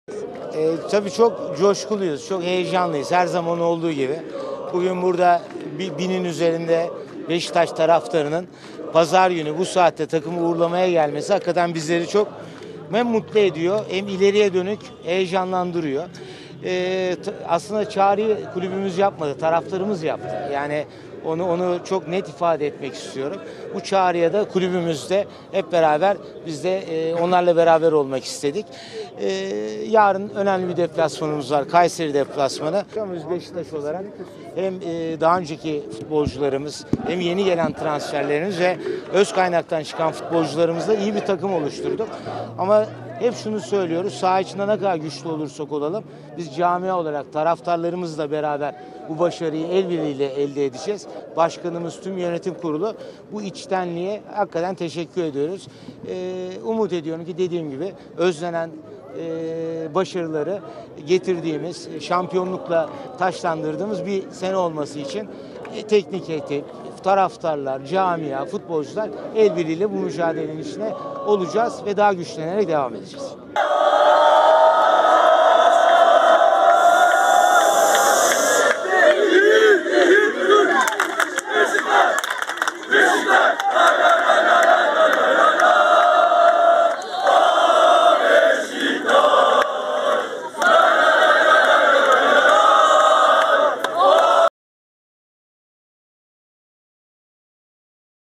Mete Vardar: Taraftarlarımızla birlikte bu başarıyı elde edeceğiz (METİN EKLENDİ)
Mete Vardar: Taraftarlarımızla birlikte bu başarıyı elde edeceğiz\r\nAli DANAŞ / İSTANBUL, (DHA)- BEŞİKTAŞ Asbaşkanı Mete Vardar, “Bugün binin üzerinde Beşiktaş taraftarının pazar günü bu saatte takımı uğurlamaya gelmesi bizleri çok mutlu etti. Saha içinde ne kadar güçlü olursak olalım biz camia olarak taraftarlarımızla birlikte bu başarıyı elde edeceğiz” dedi. \r\nBeşiktaş taraftarı, Nevzat Demir Tesisleri'ne gelerek takımı Kayserispor deplasmanına uğurladı. Beşiktaş Asbaşkanı Asbaşkanı Mete Vardar, takım tesislerden çıkmadan önce açıklamalarda bulundu. Taraftarın tesislere gelmesinden mutlu olduklarını söyleyen Mete Vardar, “Her zaman olduğu gibi çok coşkuluyuz, heyecanlıyız. Bugün binin üzerinde Beşiktaş taraftarının pazar günü bu saatte takımı uğurlamaya gelmesi bizleri çok mutlu etti. İleriye dönük heyecanlandırıyor. Aslında çağrıyı kulübümüz yapmadı, taraftarımız yaptı. Bu çağrıda biz de onlarla beraber olmak istedik. Yarın önemli bir deplasmanımız var. Lig çok zor bir lig. Her takım gücünü ortaya koymak istiyor. Biz Beşiktaş olarak hem eski oyuncularımız hem yeni gelen transferlerimiz ve öz kaynaktan çıkan futbolcularımızla iyi bir takım oluşturduk. Saha içinde ne kadar güçlü olursak olalım biz camia olarak taraftarlarımızla birlikte bu başarıyı elde edeceğiz. Bu içtenliğe teşekkür ediyoruz. Umut ediyorum ki özlenen başarıları getirdiğimiz, şampiyonlukla taçlandırdığımız bir sene olması için teknik ekip, futbolcular, camia el birliğiyle bu mücadeleyi vereceğiz” şeklinde konuştu. \r\n\r\n